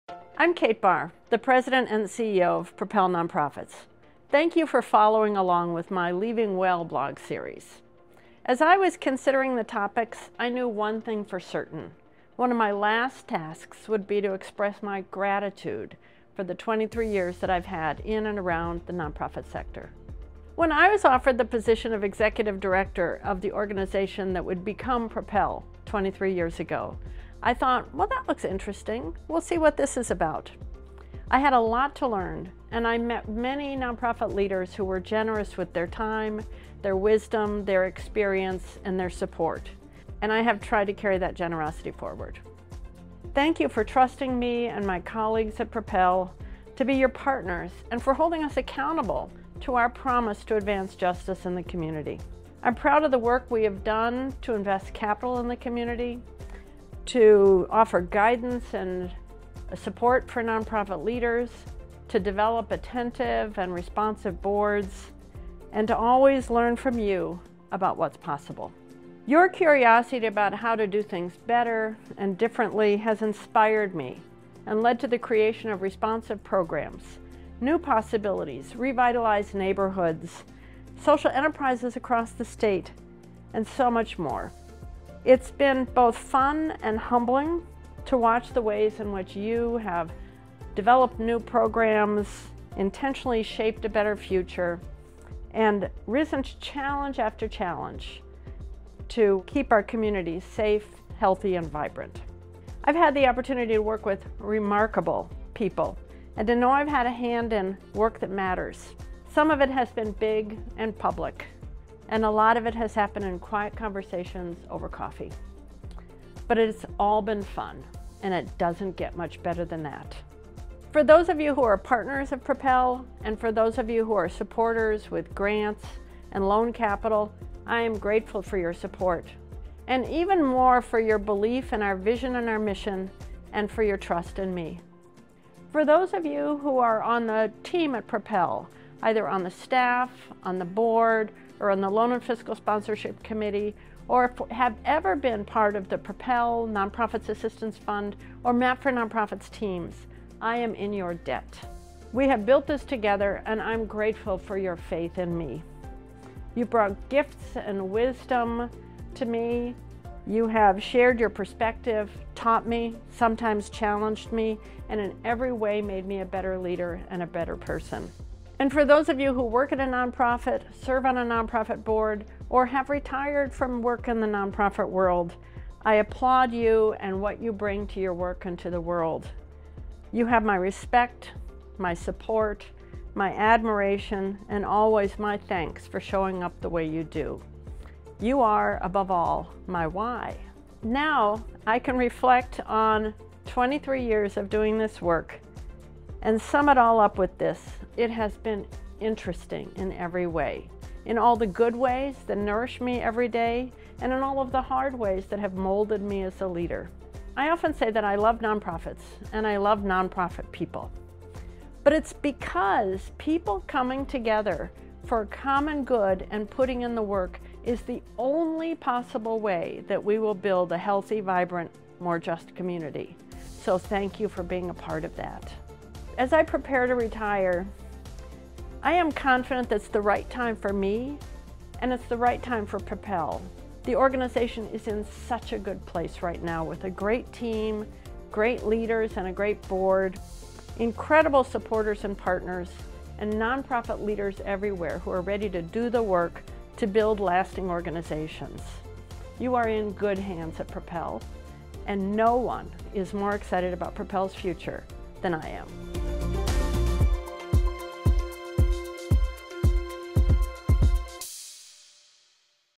Leaving Well with Kate Barr - Saying Thank You
Propel Nonprofits President & CEO Kate Barr announced her retirement in June 2023. As she wraps up her "Leaving Well" series, she says thank you to the nonprofit leaders and organizations interesting and fun. You can read the full series at Propel's